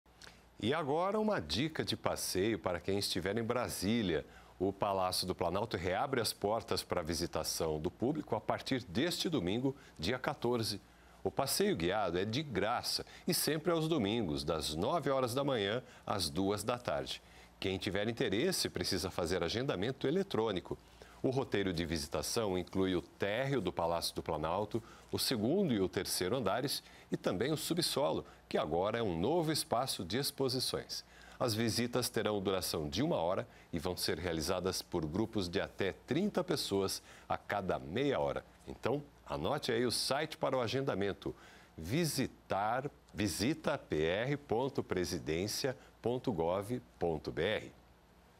PALÁCIO DO PLANALTO REABRE PARA VISITAÇÃO DO PÚBLICO
O Palácio do Planalto foi reaberto para a visitação do público. A visitação guiada é gratuita e deve ser agendada pelo site visitapr.presidencia.gov.br .